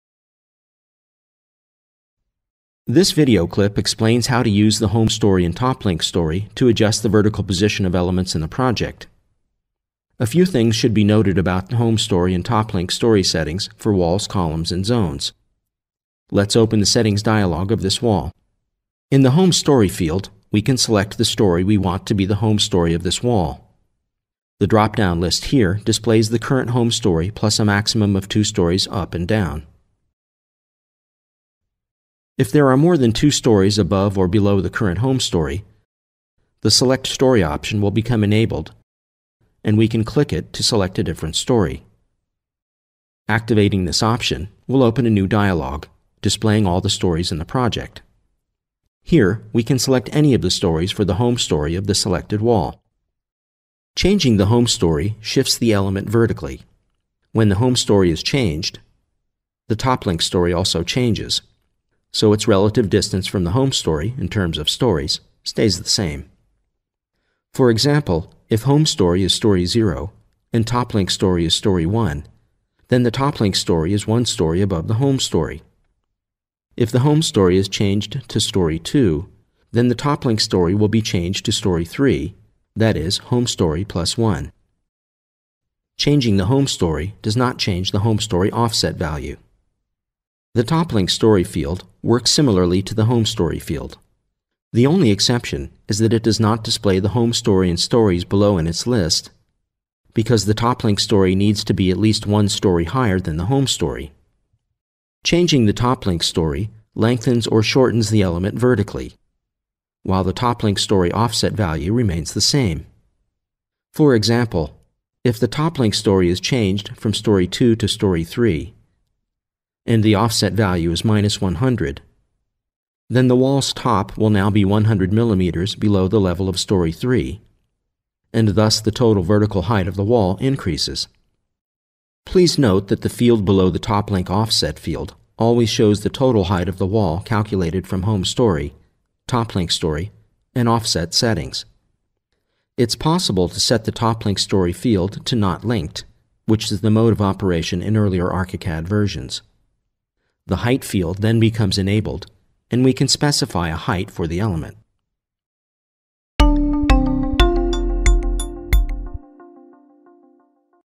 Element link in ARCHICAD: home or toplink story in settings dialog
This video clip explains how to use the home story and toplink story to adjust the vertical position of elements in the project.